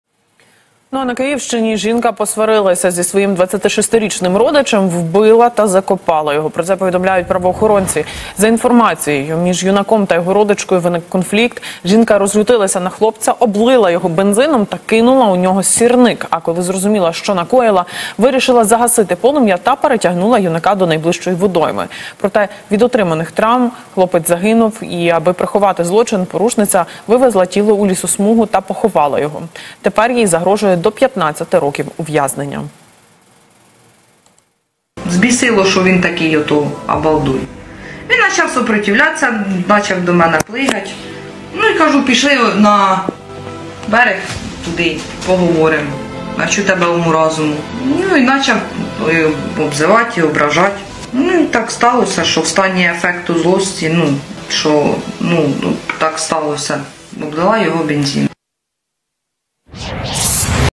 Облила бензином, підпалила та закопала труп у полі
В сюжеті  телеканалу "Київ 24" розповіли про жінку, яка облила бензином свого 26-річного родича, підпалила та закопала труп у полі. Правоохоронці Київщини затримали жінку - вбивцю та допитали.